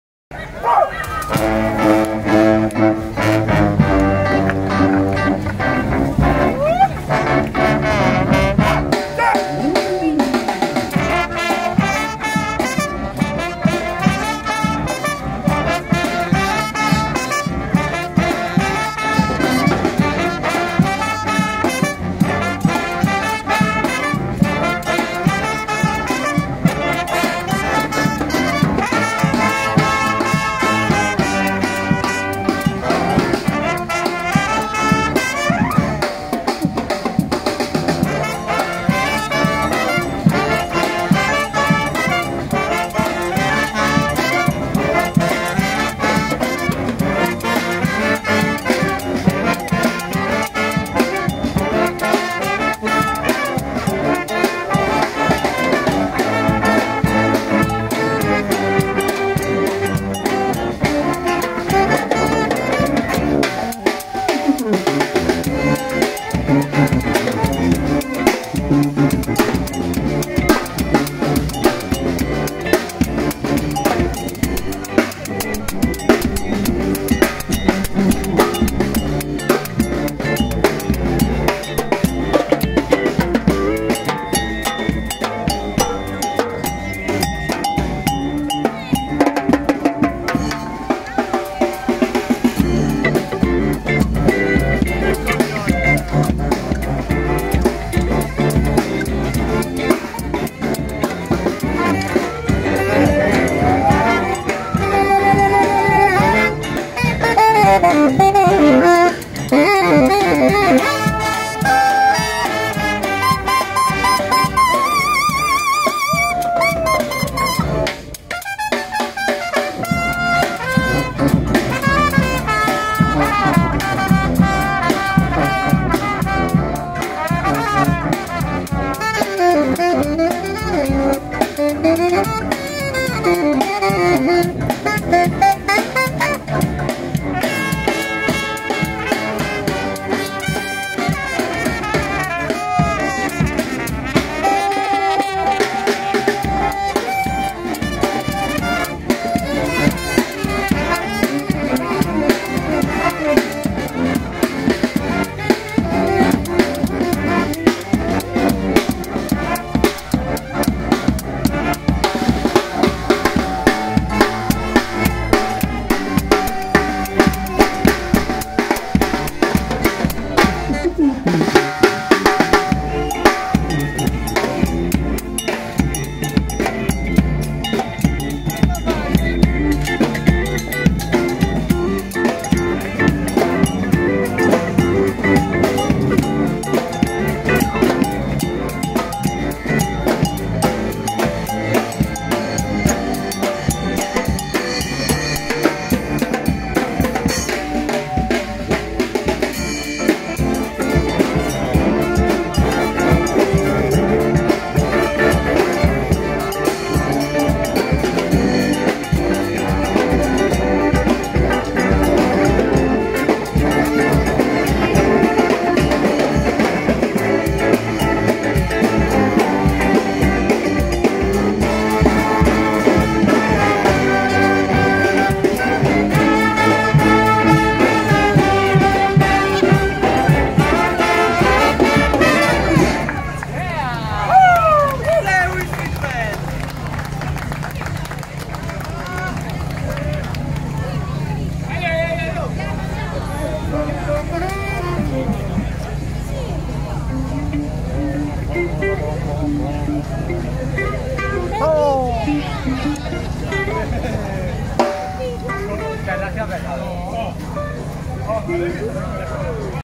SEUI STREET BAND A MONASTIR PER SAGRA DELLA PATATA VIDEO 3/4
SEUI STREET BAND A MONASTIR. Gruppo musicale di SEUI SARDEGNA. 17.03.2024